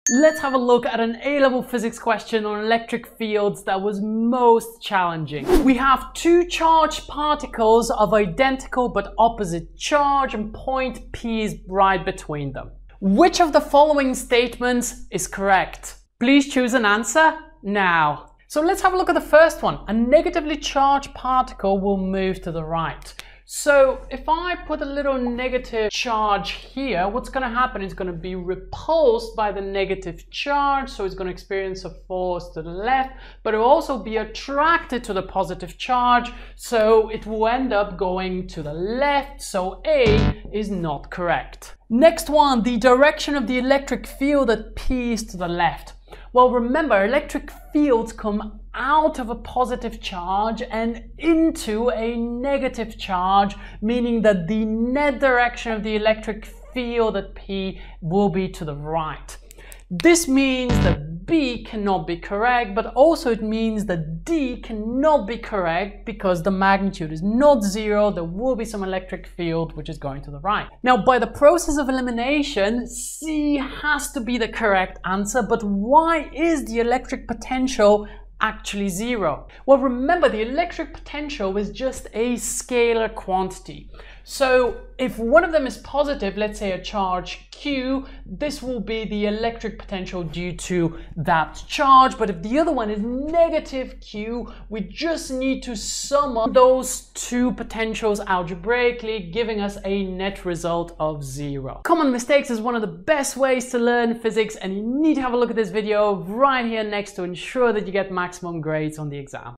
"Most Challenging" A Level Physics Question
Electric Fields Revision Question from the 2021 OCR Physics A Exploring Physics Paper 2 Solutions on Electric Fields.
According to the examiner report, Q4, Q5 and Q12 were "most challenging" and this is one of them. Here is the examiner's report
This question is excellent physics revision for all exam boards including OCR Physics A, OCR Physics B,  AQA Physics, Edexcel Physics, Cambridge International and others.